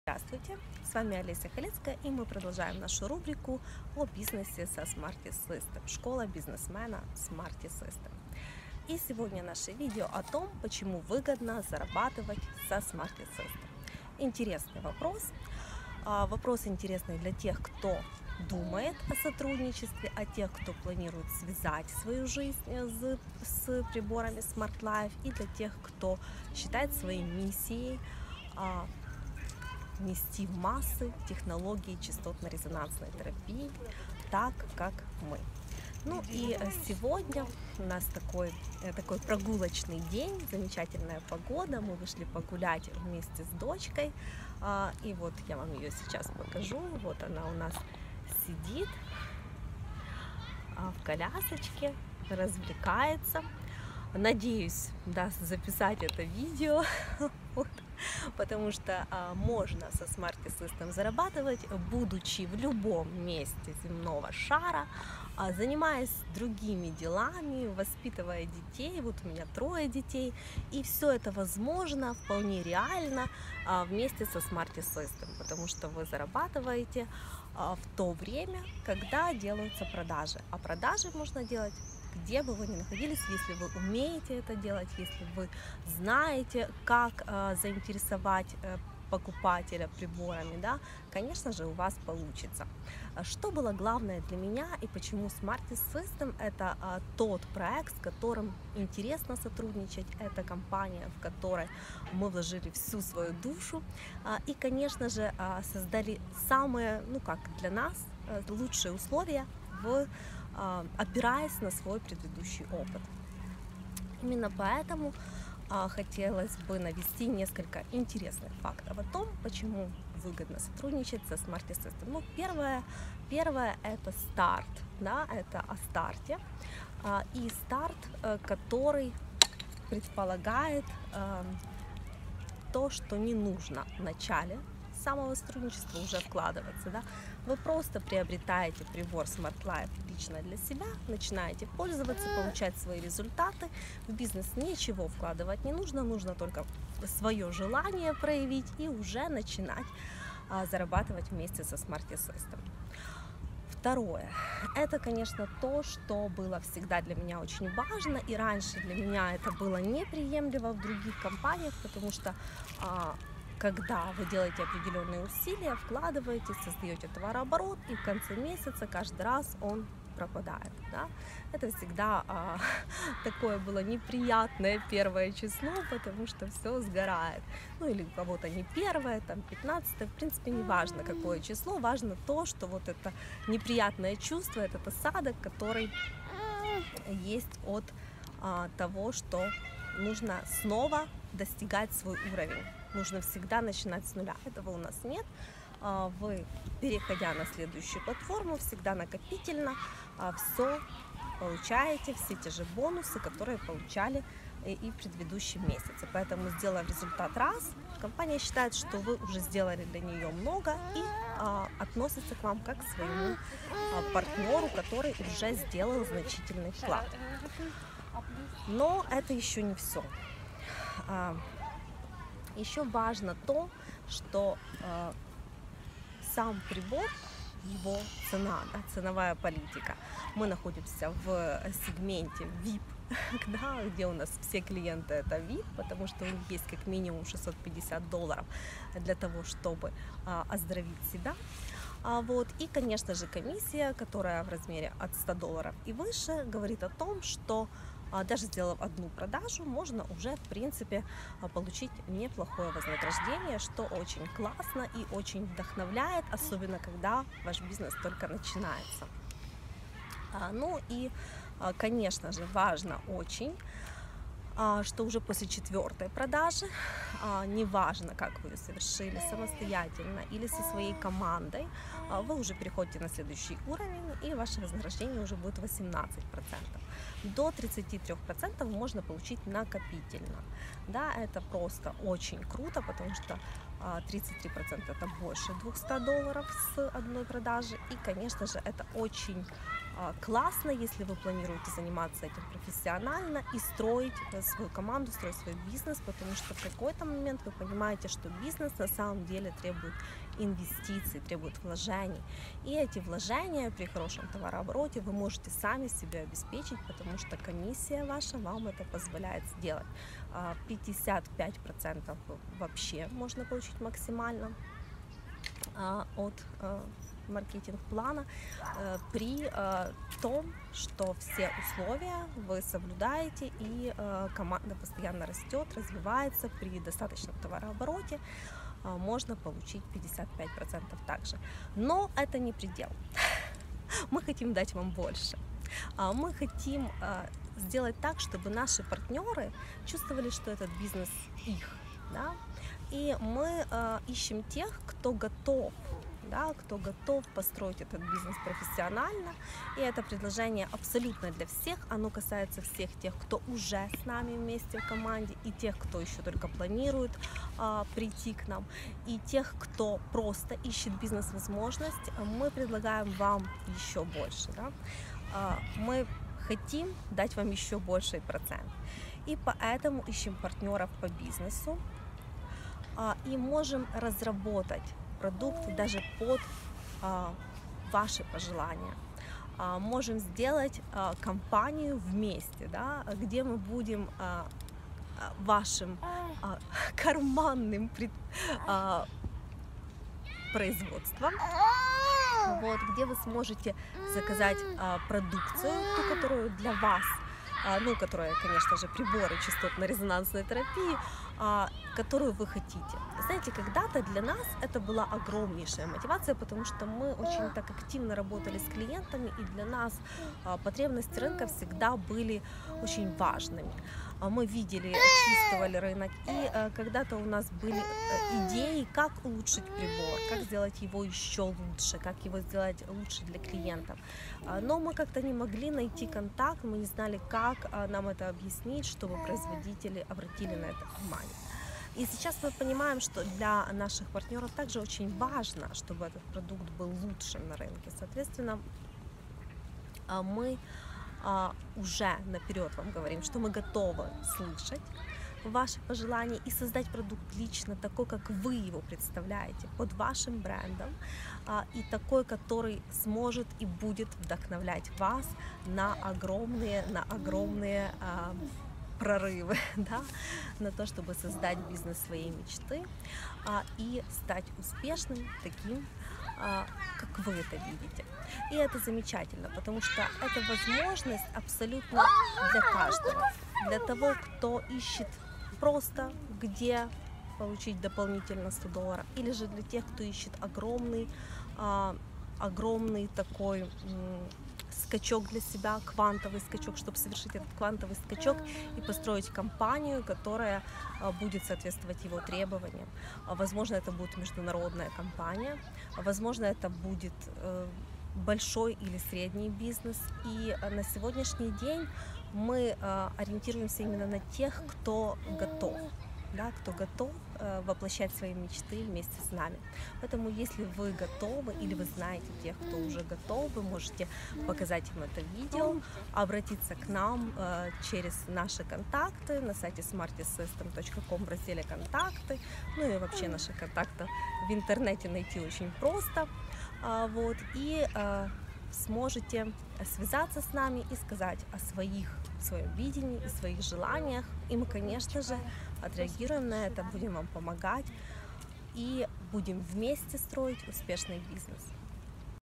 Почему выгодно зарабатывать со Smarti System?
1. Старт без вложений;
2. Невозможно потерять свой уровень вознаграждения;
3. От 100 у.е. вознаграждения с одной сделки;
4. Уже после 4-той продажи возвышение процента вознаграждения. 33% накопительно и до 55% в маркетинг;
5. Возможность дилерства- создания собственного продукта, White Label.